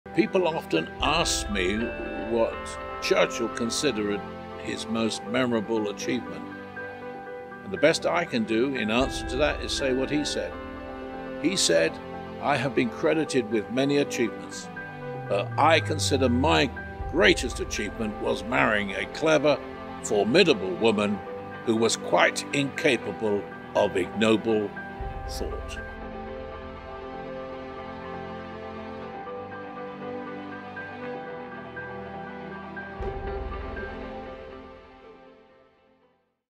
CHURCHILL starring David Payne - 1080p
David Payne discusses what Churchill thought was his greatest achievement.